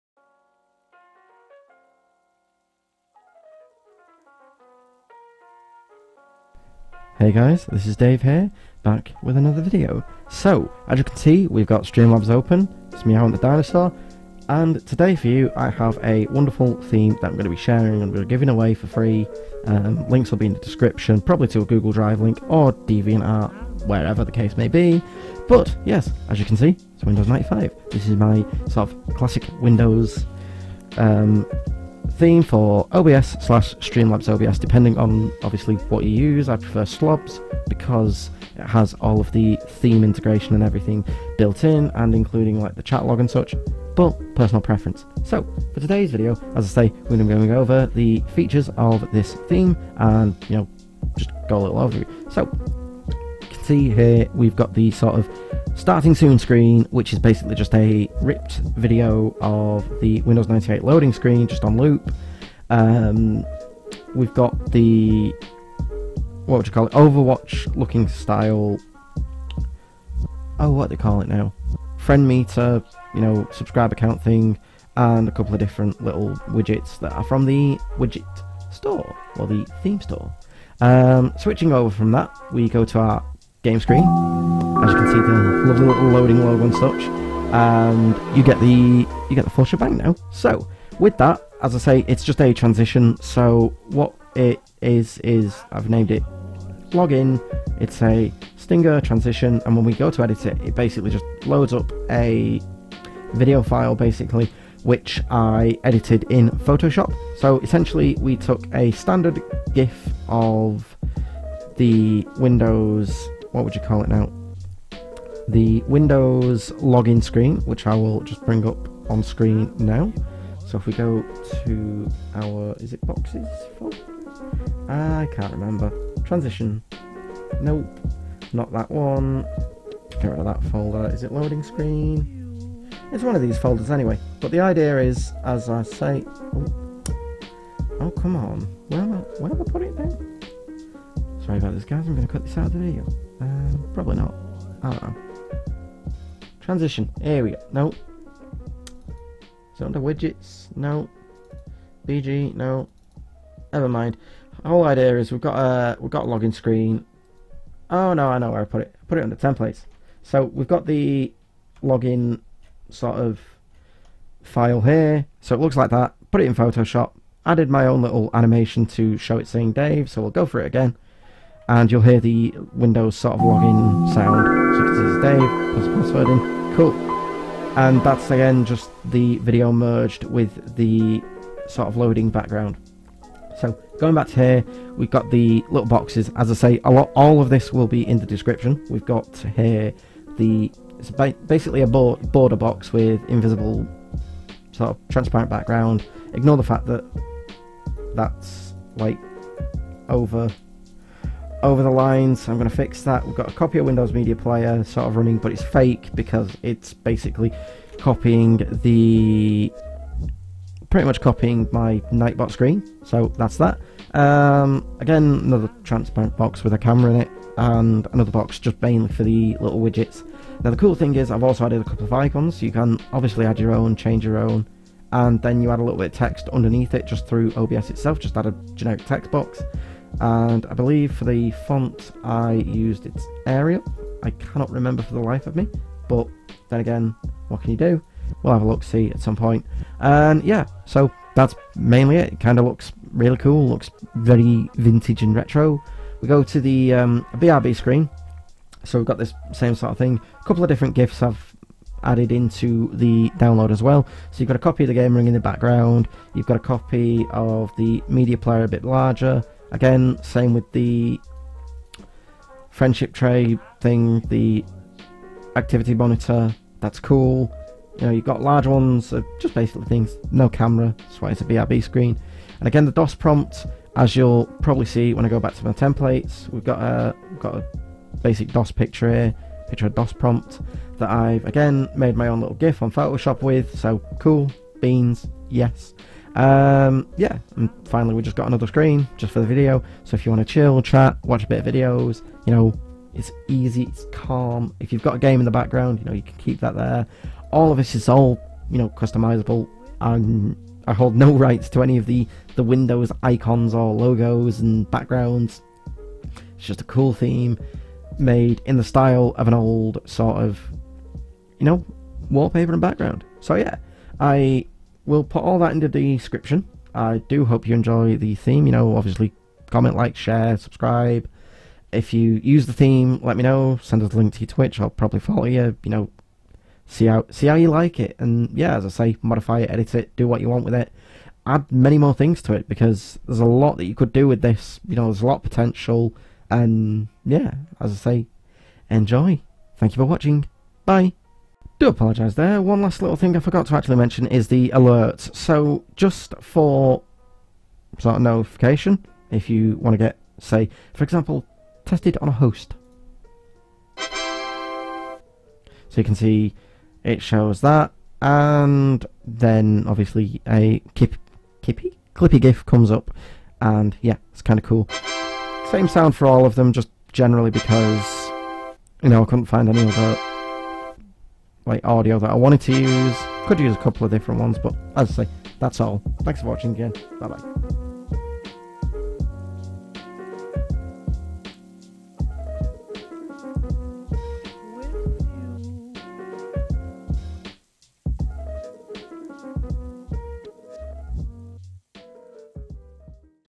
FREE LIVE STREAM OVERLAY!! CLASSIC WINDOWS 95/98 TWITCH OBS THEME
Enjoy my free classic windows style theme for twitch obs/slobs provided are templates and FREE resources to use within the archive to customise your stream to the fullest classic potential.

HOPE YOU ALL ENJOY THIS THEME - If you do use this please let me know your twitch/yt account and ill check you out